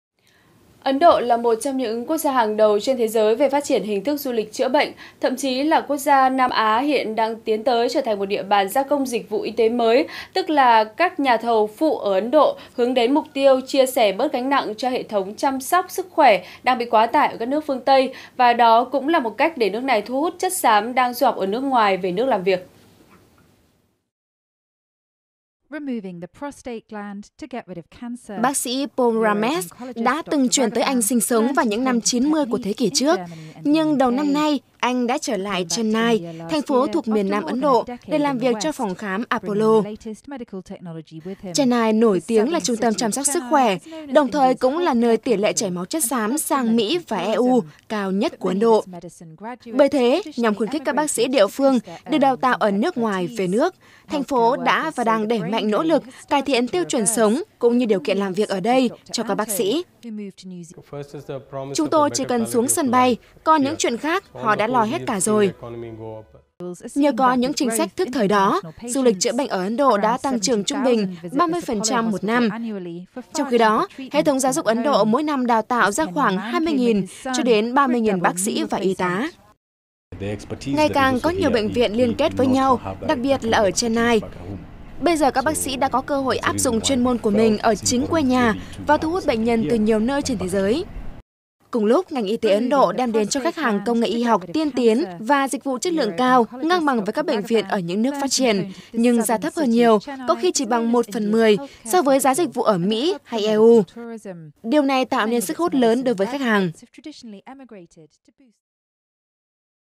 VTC14_Ấn Độ: Nhiều bác sĩ về nước làm việc sau du học
(Truyền hình VTC14) - Ấn Độ là một trong những quốc gia hàng đầu trên thế giới về phát triển hình thức du lịch chữa bệnh. Thậm chí, quốc gia Nam Á hiện đang tiến tới trở thành một địa bàn "gia công dịch vụ y tế mới", tức là các nhà thầu phụ ở Ấn Độ hướng đến mục tiêu chia sẻ bớt gánh nặng cho hệ thống chăm sóc sức khỏe đang bị quá tải ở các nước phương Tây. Đó cũng là một cách để nước này thu hút chất xám đang du học ở nước ngoài về nước làm việc.